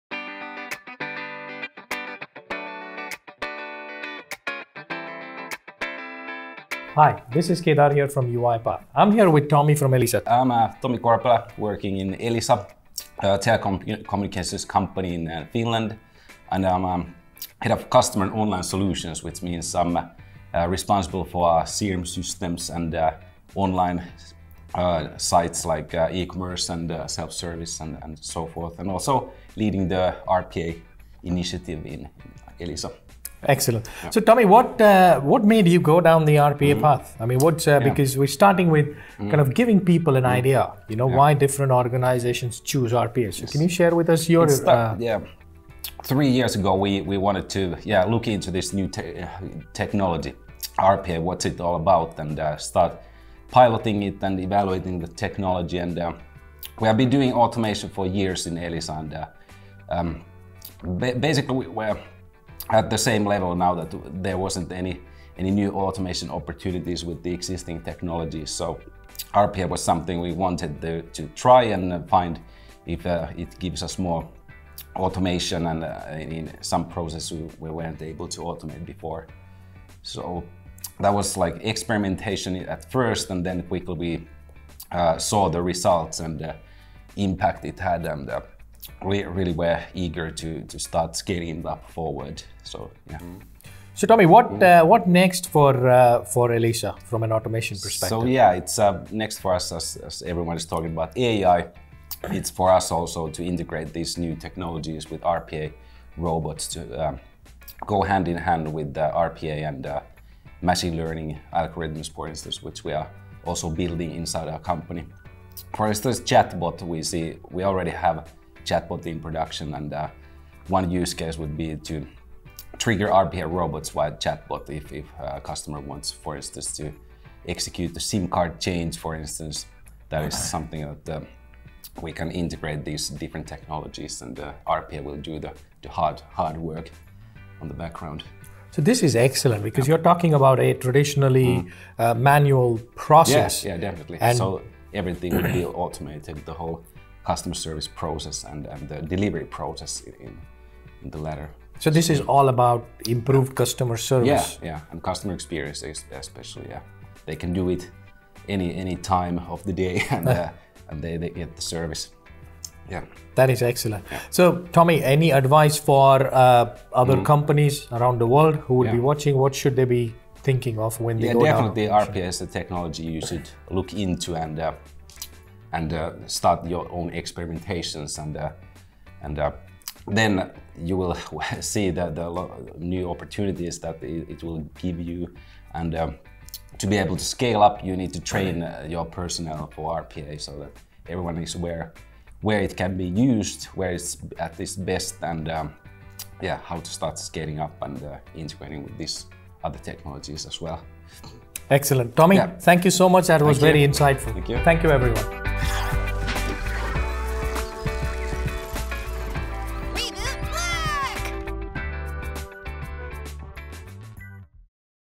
Automation Revealed: Elisa combines RPA and Machine Learning to improve customer service
Welcome to a new episode of Automation Revealed with Kedar Dani. Kedar caught up with Tomi Korpela who leads the RPA initiative at Elisa. Tomi disused Elisa’s journey with RPA and their integration of Machine Learning to not only improve customer experience, but service as well. He stressed the importance of testing and experimentation during your journey into RPA and how it is an ever-evolving process.

Watch the video for Tomi’s full thoughts.